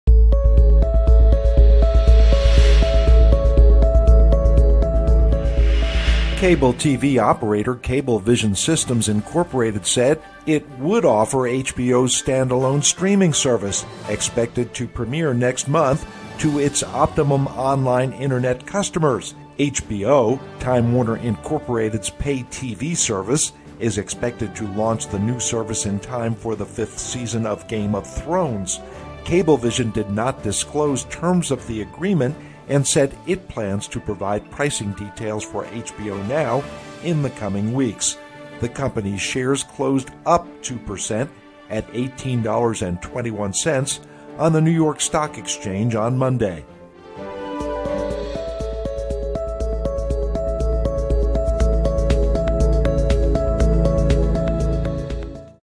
Cablevision to Offer HBO NOW to Online Customers
Cable TV operator Cablevision Systems Inc said it would offer HBO's standalone streaming service, expected to premiere next month, to its Optimum Online internet customers.